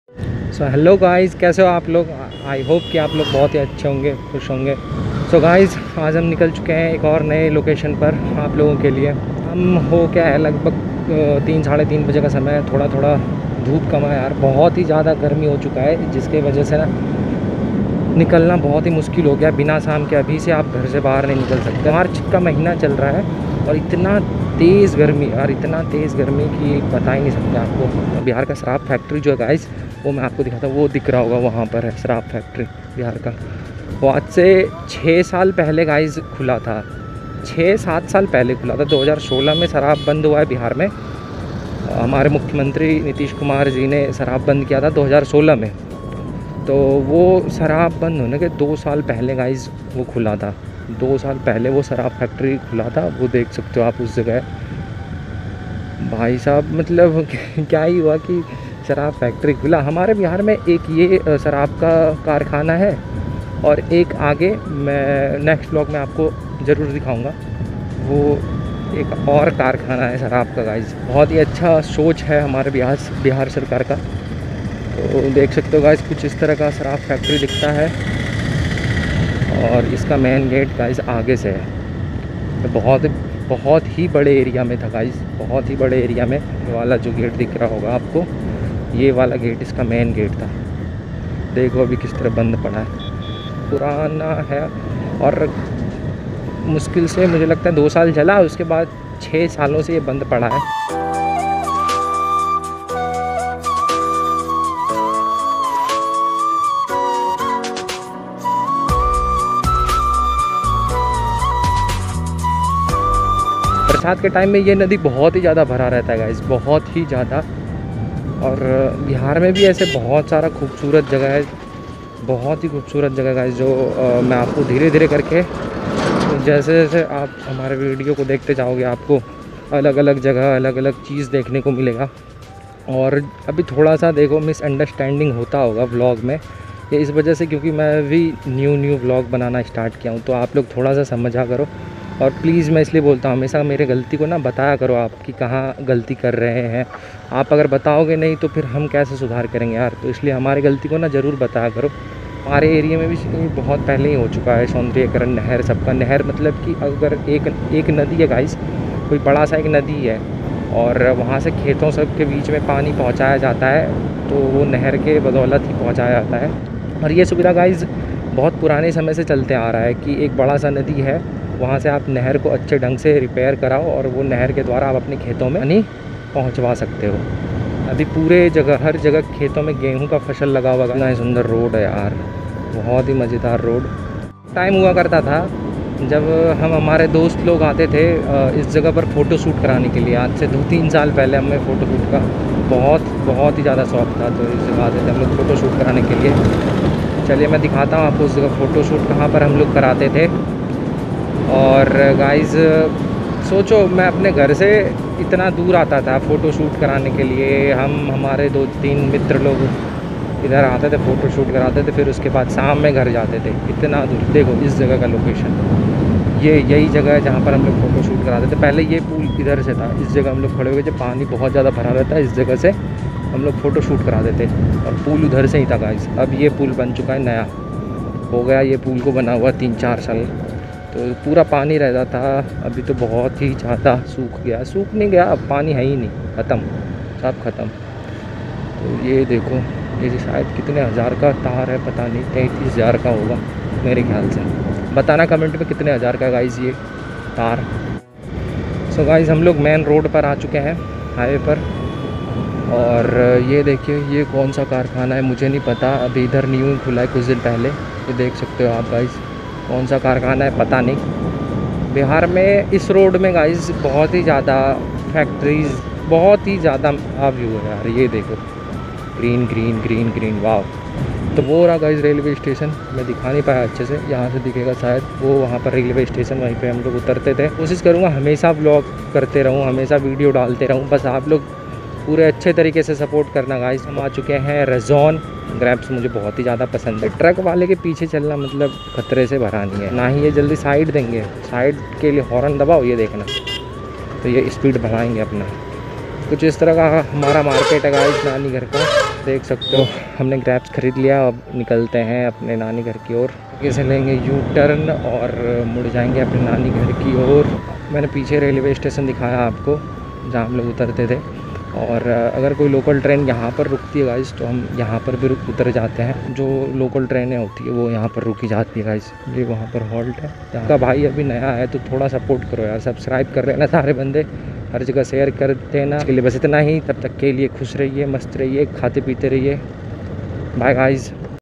Ye Band Kab Hua🤔 || 2 Factory🏭 Close
Good Morning
Hello Guys, My Name Anshu Jha

About This Video :- road pr jaate time 2 sarab factory dikh gaya.humlog ka photo shoot. main road pr ek factory. railway station dikh gaya.nani ghar jaa raha hu

Instagram on👉 anshu_bhai11